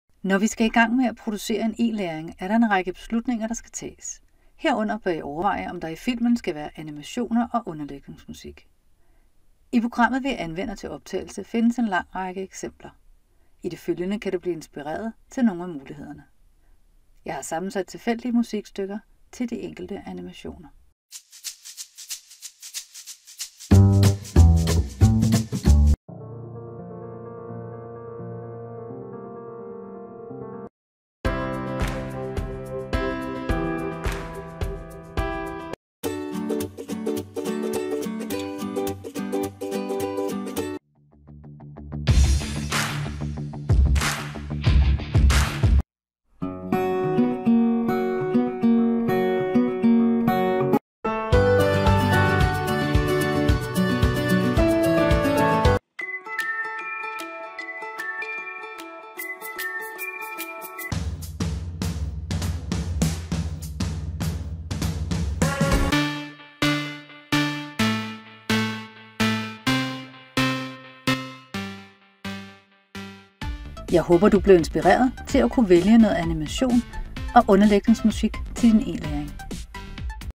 E-Learning Universe, Eksempler på animation og underlægningsmusik
Denne video viser nogle eksempler på animationer og underlægningsmusik til inspiration for din kommende E-Læring.